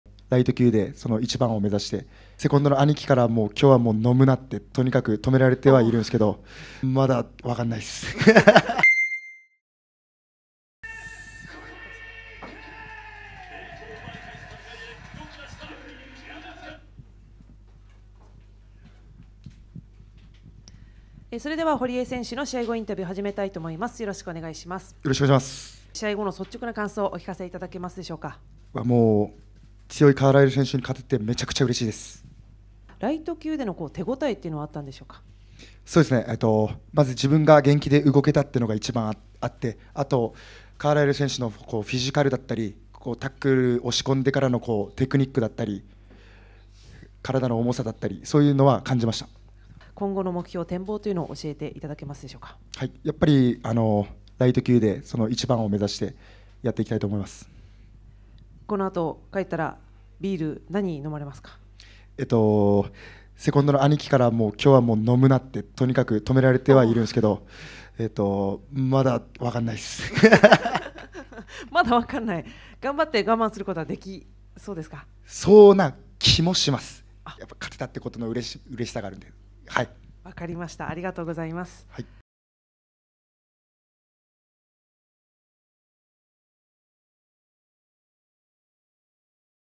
【RIZIN】 堀江圭功、スパイク・カーライルに勝ち喜び爆発も“勝利の美酒”はお預け？ 『RIZIN.44』試合後インタビュー
『RIZIN.44』が24日、さいたまスーパーアリーナで行われ、クレベル・コイケや金原正徳ら出場選手がアツい闘いを繰り広げた。